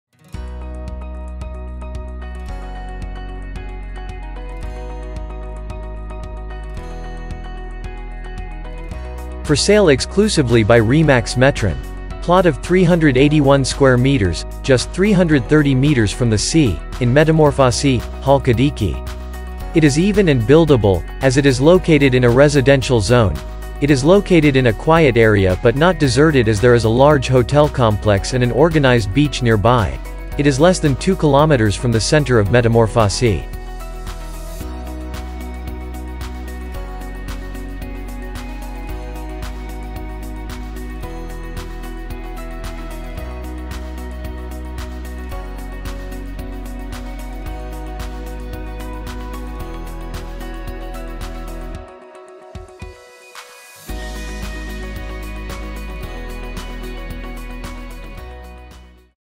Οικόπεδο 381 τ.μ., μόλις 330 μέτρα από τη θάλασσα, στην Μεταμόρφωση Χαλκιδικής!
Πωλείται αποκλειστικά από τη RE/MAX Metron!

Οικόπεδο 381 τ.μ., μόλις 330 μέτρα από τη θάλασσα, στην Μεταμόρφωση Χαλκιδικής! Είναι άρτιο και οικοδομήσιμο, καθώς βρίσκεται σε οικιστική ζώνη. Βρίσκεται σε ήσυχη περιοχή αλλά όχι ερημική καθώς κοντά βρίσκεται μεγάλη ξενοδοχειακή μονάδα και οργανωμένη παραλία.  Απέχει λιγότερο από 2 χιλιόμετρα από το κέντρο της Μεταμόρφωσης.
Απόσταση από χωριό(m):1.860  Πόλη(m):7.460 Θάλασσα(m):330 Αεροδρόμιο(m):60.680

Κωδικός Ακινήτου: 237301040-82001

RE/MAX Metron Halkidiki
Δημ.Τζαβούρα 9, 63200
Νέα Μουδανιά, Χαλκιδική

For Sale exclusively by RE/MAX Metron!

Plot of 381 sq.m., just 330 meters from the sea, in Metamorfosi, Halkidiki! It is even and buildable, as it is located in a residential zone. It is located in a quiet area but not deserted as there is a large hotel complex and an organized beach nearby. It is less than 2 kilometers from the center of Metamorfosi.

Distance from village(m):1.860,00 Town(m):7.460,00 Sea(m):    330,00 Airport (m):60.680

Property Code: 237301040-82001

RE/MAX Metron Halkidiki
9 Dim. Tzavoura, 63200
Nea Moudania, Chalkidiki